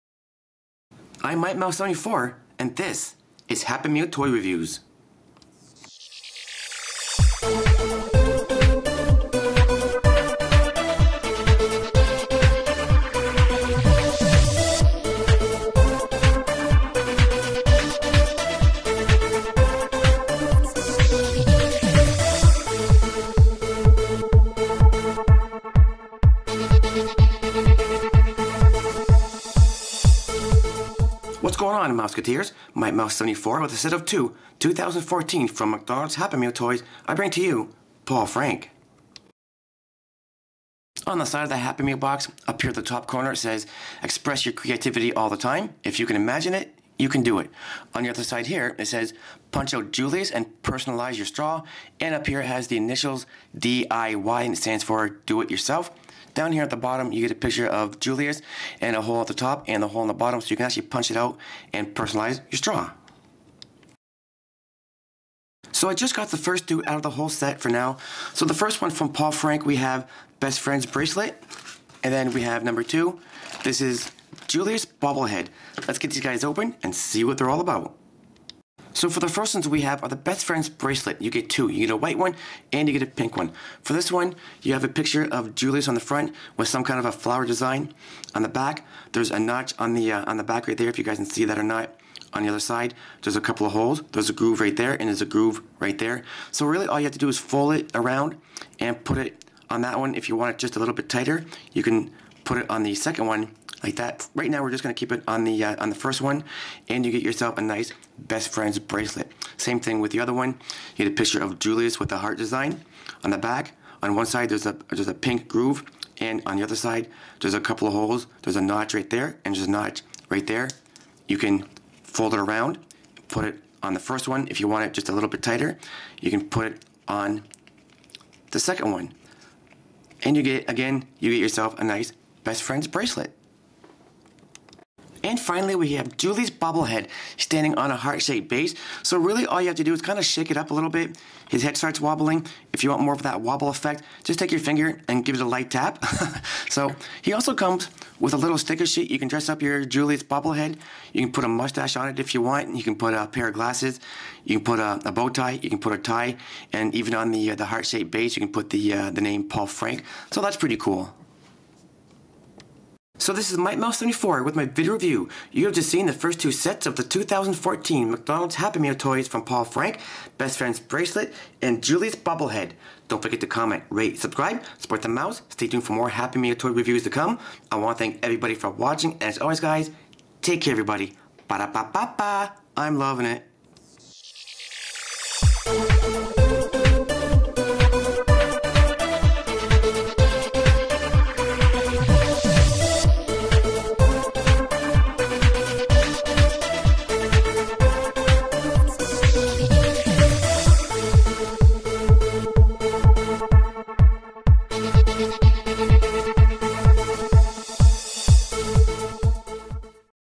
SET OF 2 2014 MCDONALDS HAPPY MEAL TOYS 1 & 2 PAUL FRANK TOY REVIEW
Hey everyone. here is my video review of the first 2 sets of the 2014 McDonalds happy meal toys Paul Frank! Let me know what you think! Thanxs for watching everybody! =D